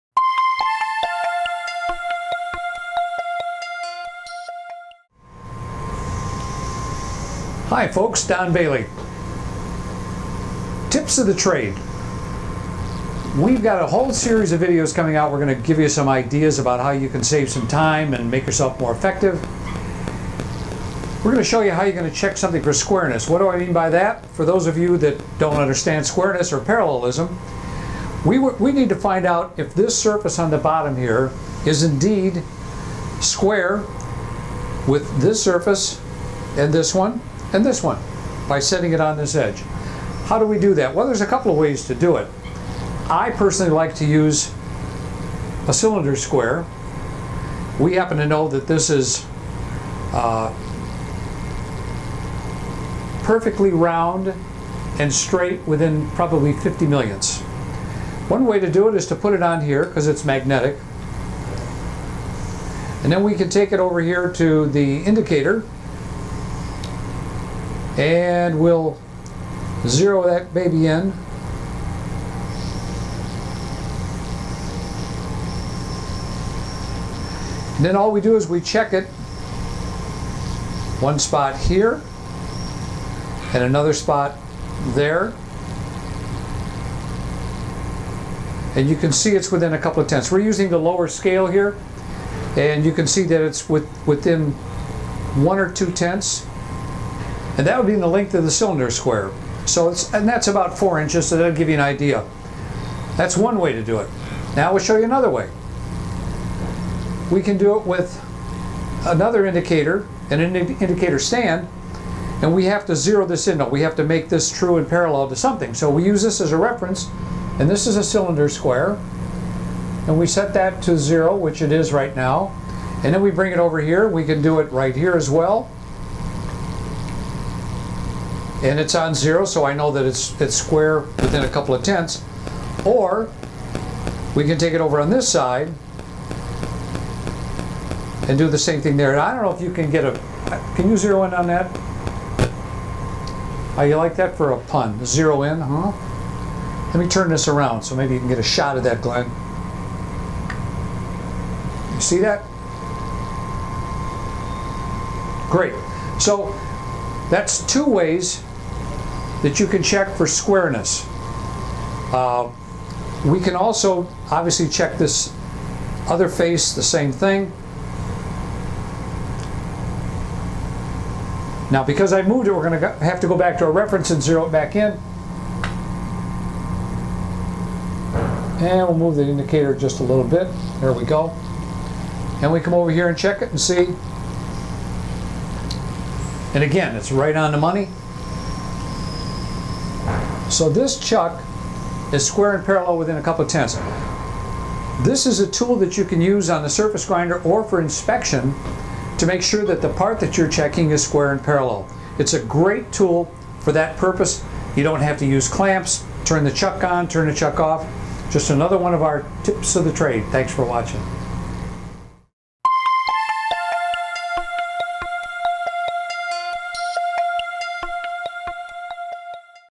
How to Check and Measure Squareness - Part 2
Don Bailey discussing How To Check and Measure Squareness Part 2